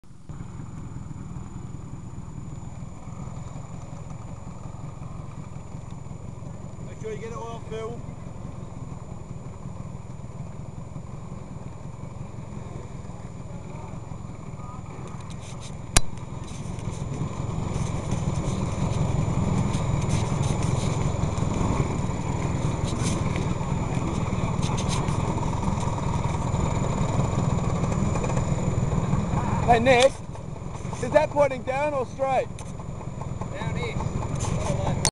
Australian National Classic Scooter Rally 2009 Helmet Cam 8
The Barossa National Classic Scooter Rally! Raw helmet camera footage from the ANSR 2009 in Adelaide